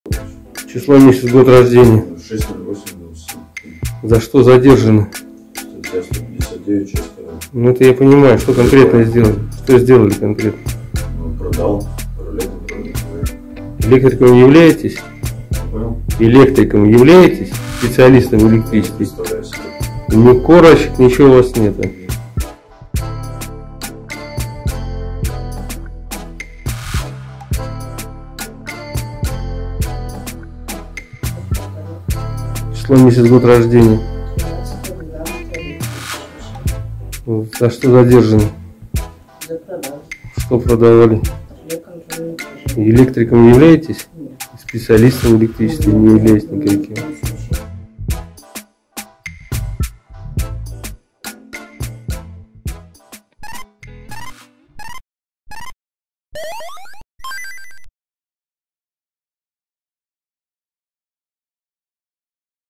Бдительный житель Луги помог задержать лже-электриков
Преступники убедили пенсионерку купить у них реле для контроля за перепадами напряжения в электросети и оплатить их установку. Сумма ущерба составила 21 тысячу рублей, сообщили в пресс-службе регионального МВД.